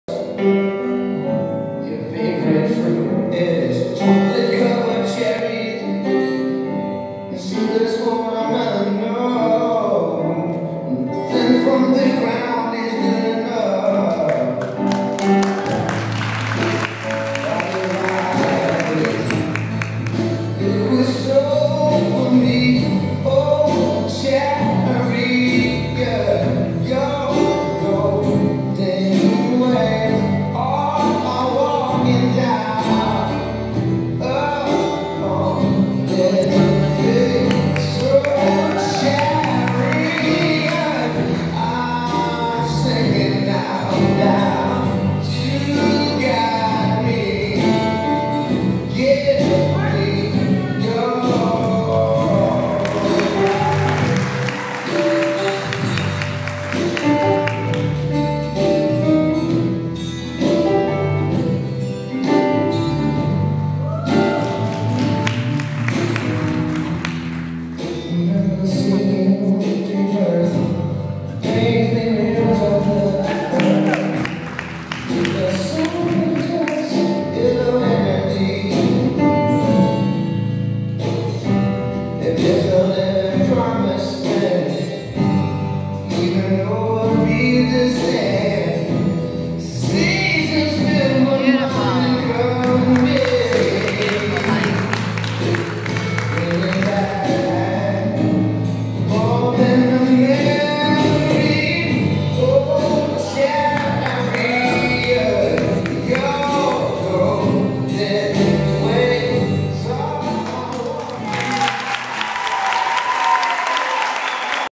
Sabrina Woods Guest Performance at Miss Georgia
Sabrina Woods Guest Performance at Miss Georgia Competition... I think I only caught half of her performance. :(